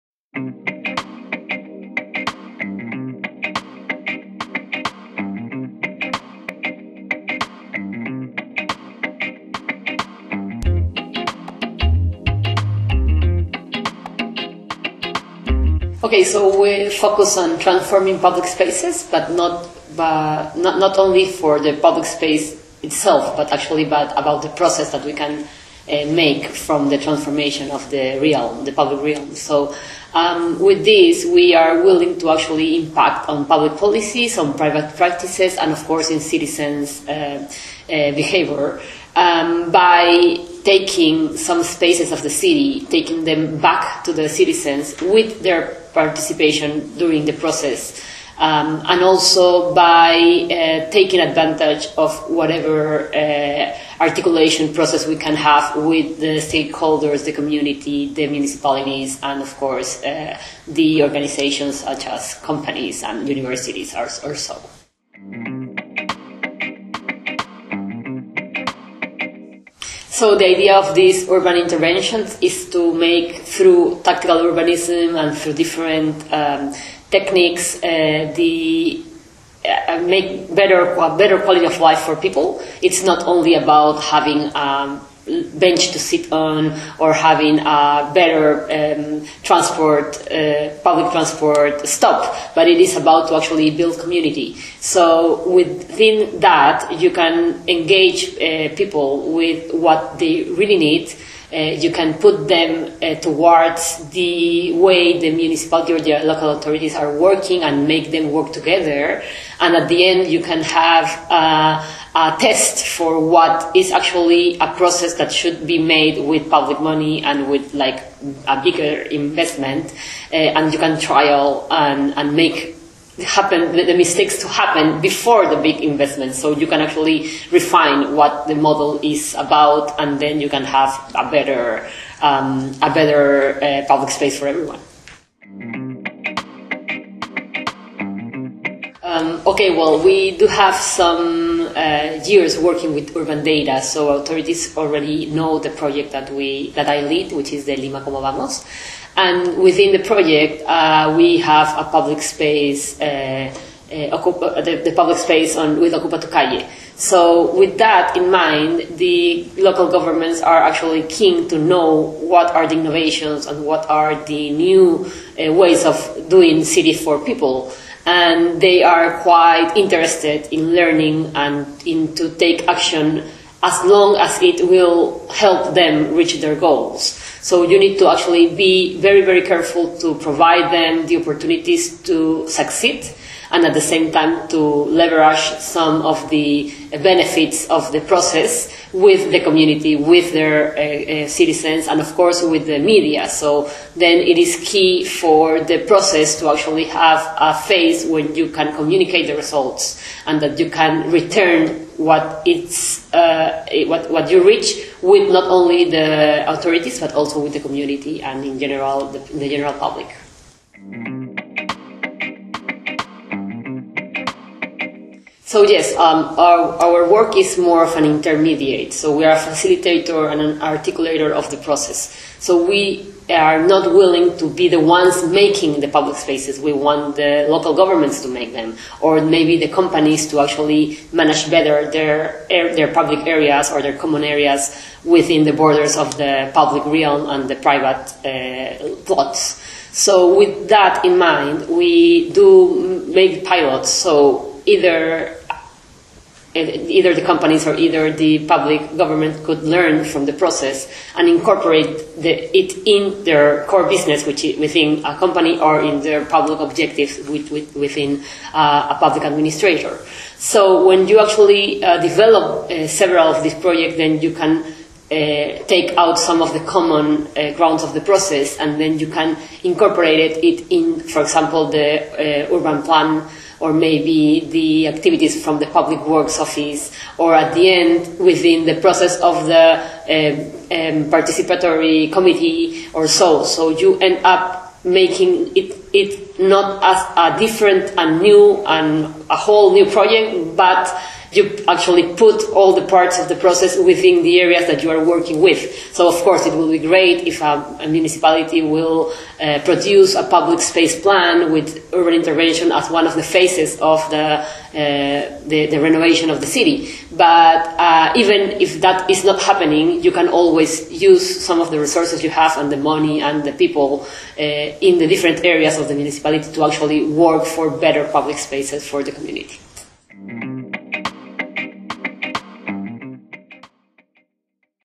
Mariana Alegre: "It is about building community"
At the Bonn Symposium 2019, we talked with Mariana Alegre, Executive Director of Ocupa tu Calle, about transforming public spaces through citizen-led micro-interventions in South America.

Watch the video and learn how to create better public spaces for everyone.